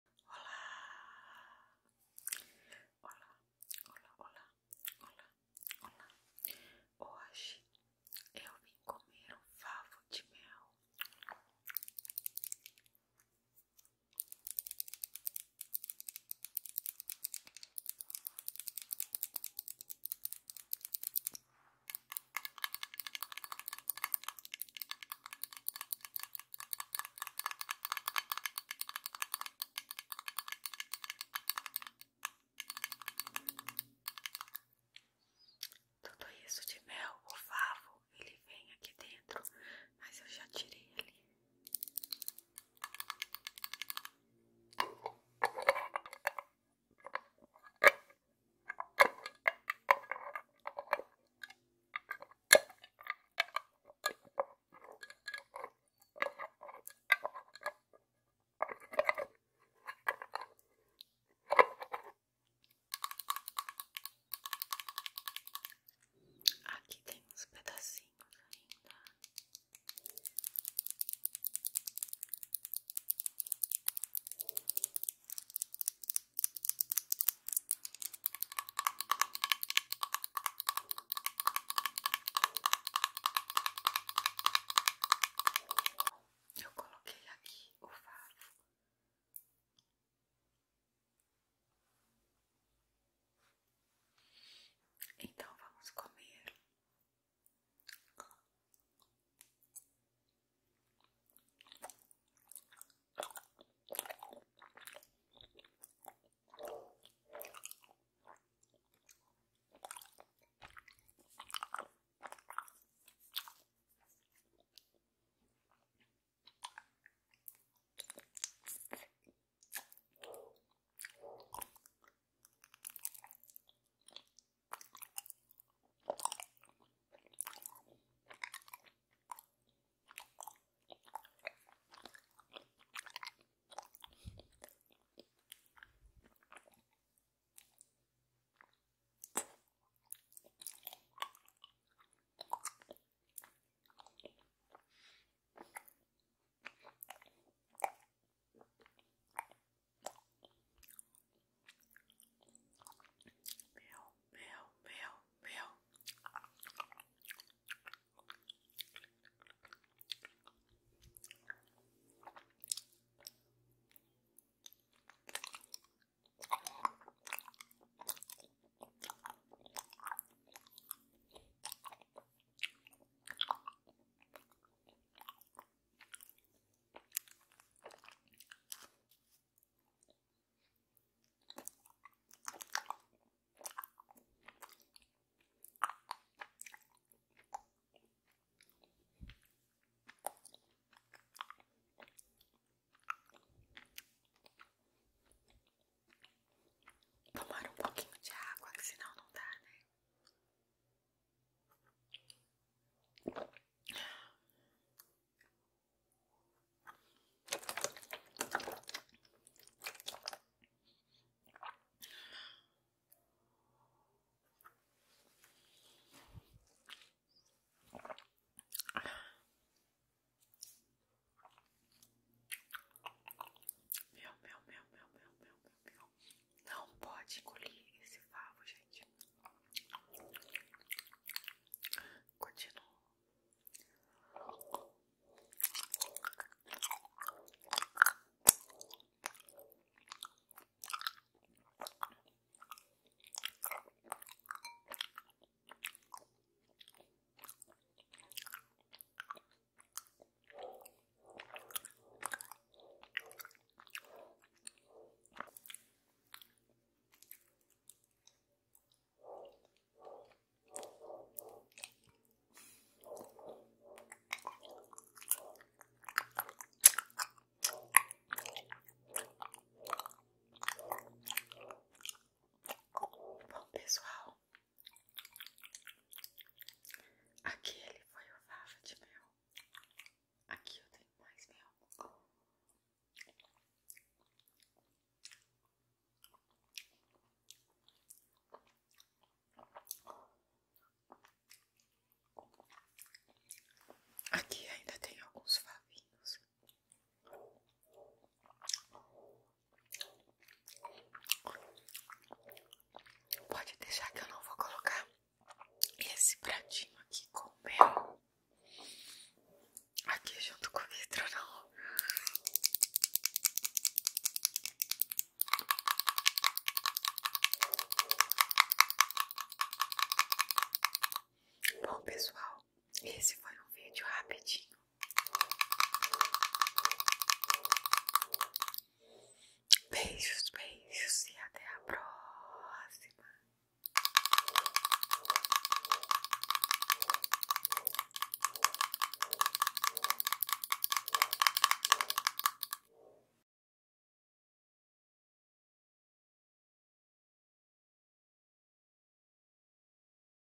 ASMR COMENDO FAVO DE MEL 🍯 SONS DE MASTIGACAO
ASMR COMENDO FAVO DE MEL
ATENÇÃO: favo de mel nao pode ser ingerido.

🌏 ASMR BRASILEIRO ✔

🎧 ASMR PARA VOCE RELAXAR E DORMIR 😴

✅ INSCREVA- SE
✅ COMPARTILHE

MUITO OBRIGADA 💟